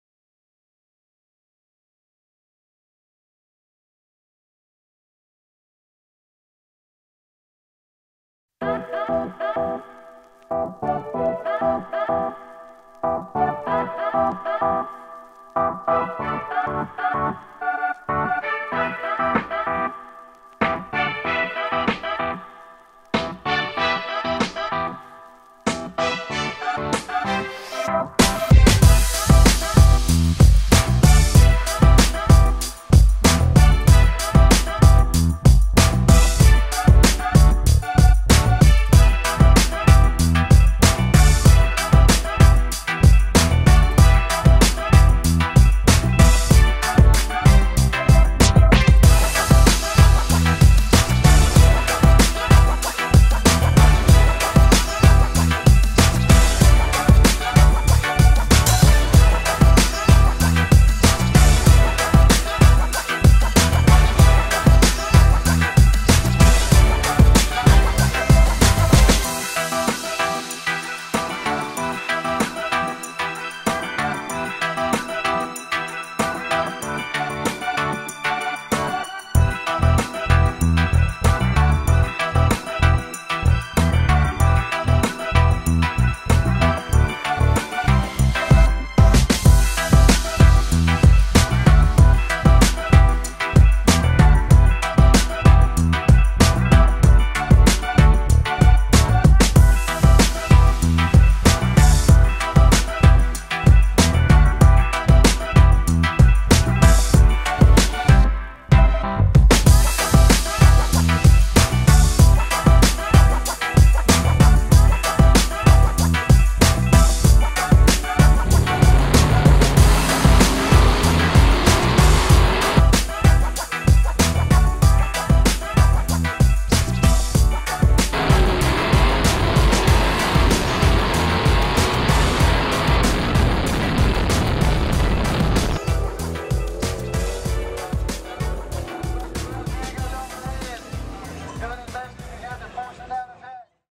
Derek Shamblin - Dodge Hellcat at KD Midnight Madness 4 - 08-27-2022
Derek Shamblin takes his Dodge Hellcat down the track at KD Motorsports Park in South Webster, OH, as part of KD's Midnight Madness 4 event on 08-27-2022.